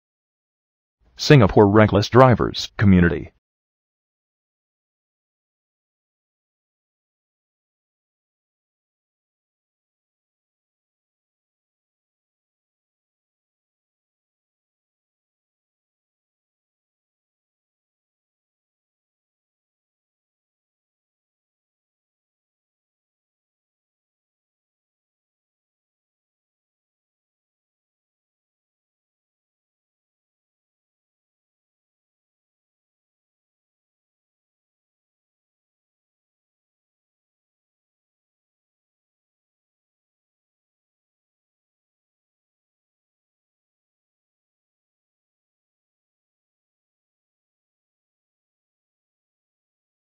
[SRD Community] The Car Door Knock Down
Motorcyclist knocked down by a car door unexpectedly. Do check if vehicle surrounding is clear before opening the car door. And it is dangerous to open the car door in the middle of the road. Drivers/riders please take caution whenever on the road.

CS Lee
Singapore Reckless Drivers Community